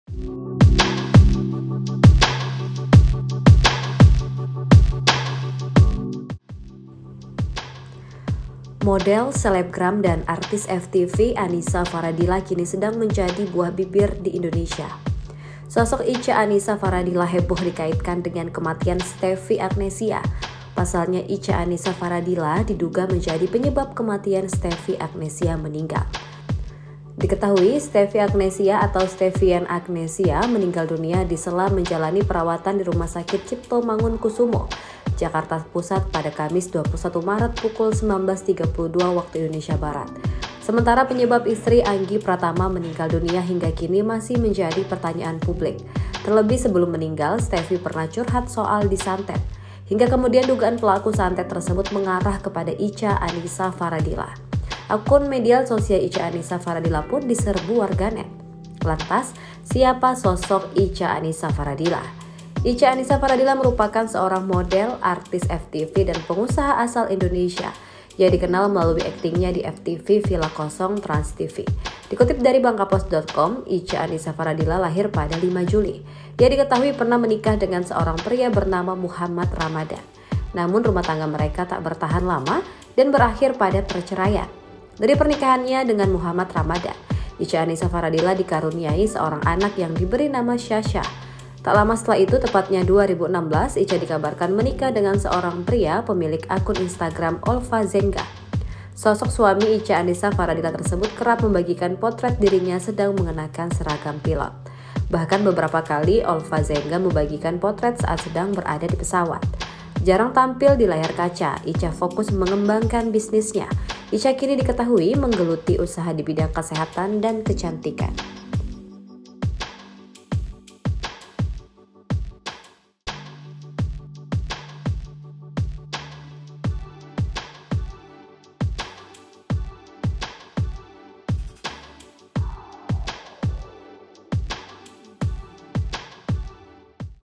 SOSOK Icha Annisa, Wanita Yang Dituding Santet Stevie Agnecya Hingga Meninggal
BANGKAPOS.COM -

 Model, selebgram dan artis FTV Annisa Faradila kini sedang menjadi buah bibir di Indonesia.

Sosok Icha Annisa Faradila heboh dikaitkan dengan kematian Stevie Agnecya.

Pasalnya, Icha Annisa Faradila diduga menjadi penyebab kematian Stevie Agnecya.

Diketahui, Stevie Agnecya atau Stevianne Agnecya meninggal dunia disela menjalani perawatan di RS Cipto Mangunkusumo, Salemba, Jakarta Pusat, pada Kamis (21/3/2024) pukul 19.32 WIB.